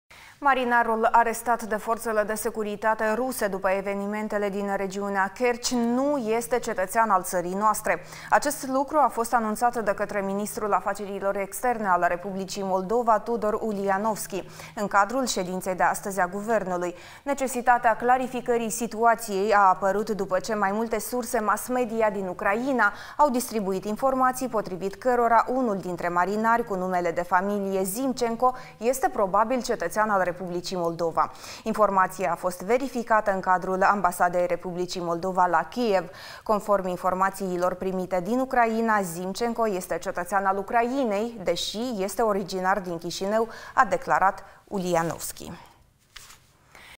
VIDEO // MARINARUL ARESTAT DE FORȚELE DE SECURITATE RUSE NU ESTE MOLDOVEAN
Marinarul arestat de forțele de securitate ruse, după evenimentele din regiunea Kerci, nu este cetățean al țării noastre. Acest lucru a fost anunțat de către ministrul Afacerilor Externe al Republicii Moldova, Tudor Ulianovski, în cadrul ședinței guvernului de astăzi.